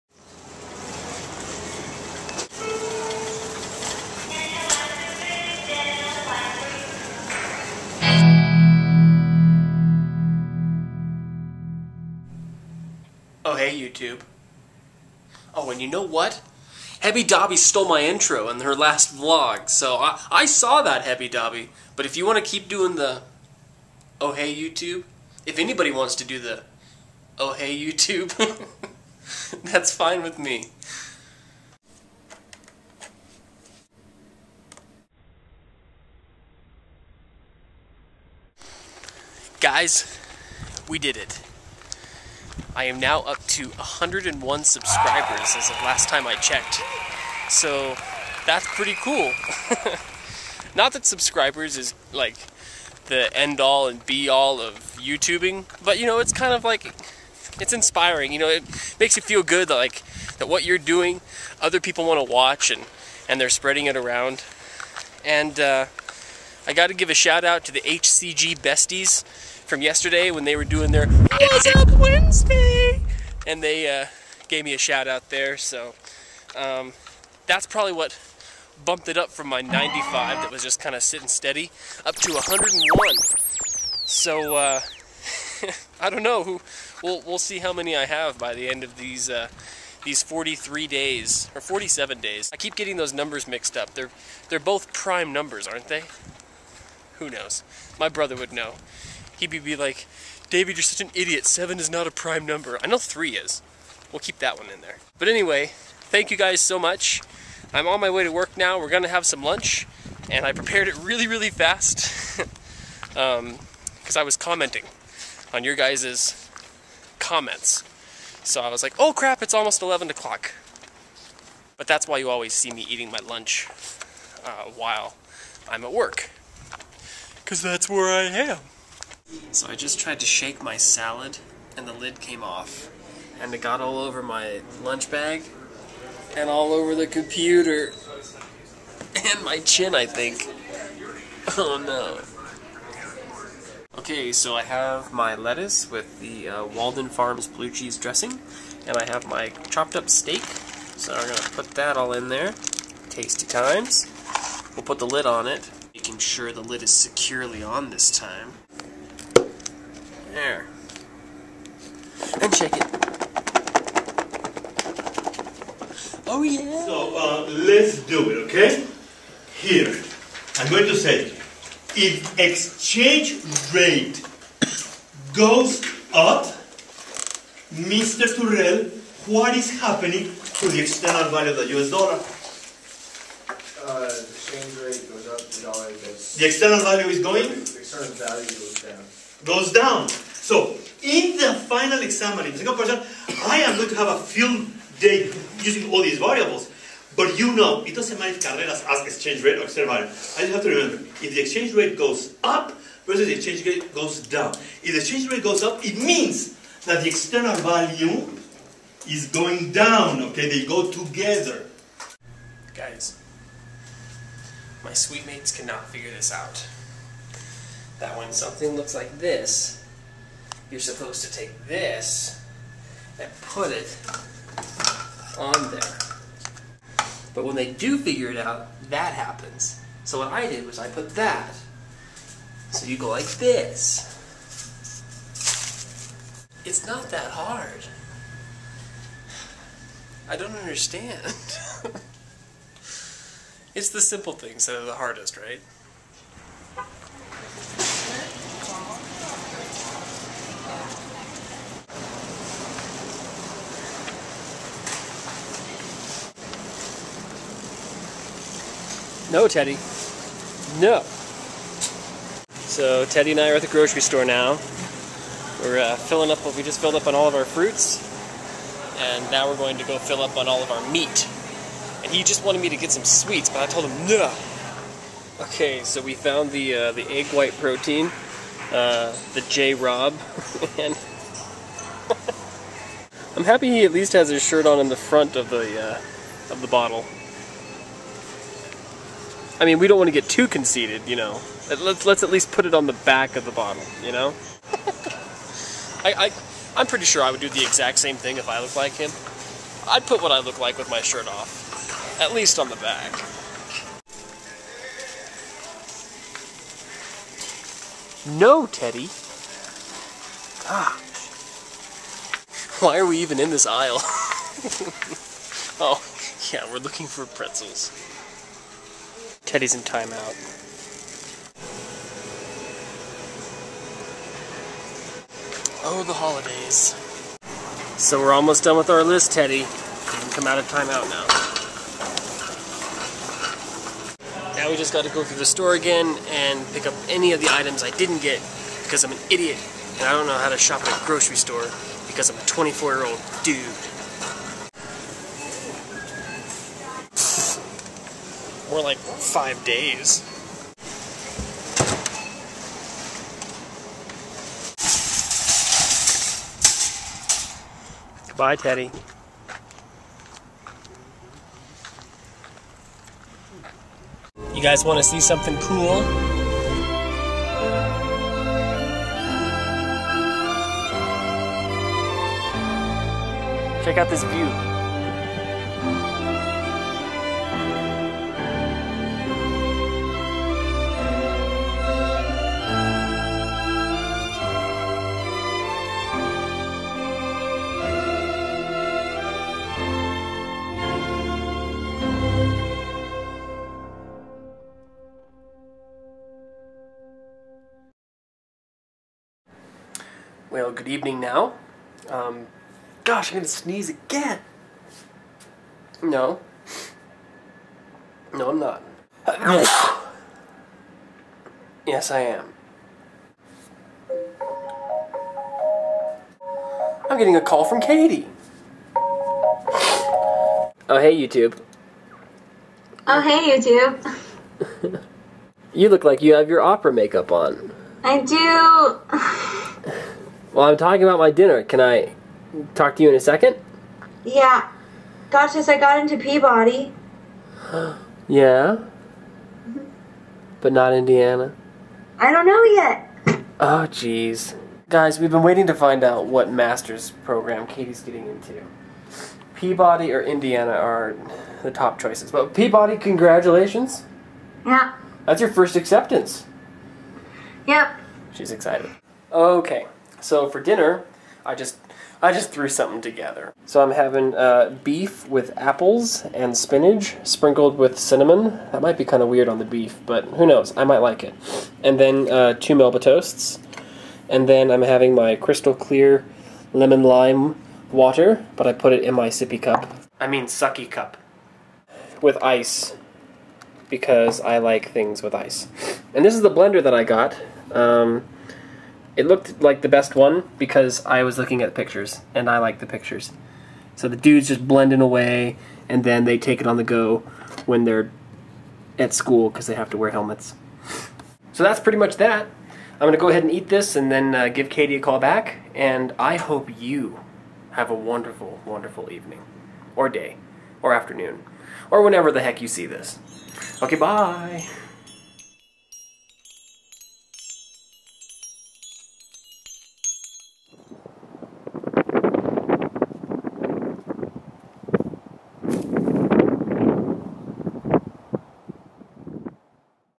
Runkman44's HCG Diet Day 11
I take you to class, the store, the top of a mountain, and even the moon! Ok...well maybe not the moon!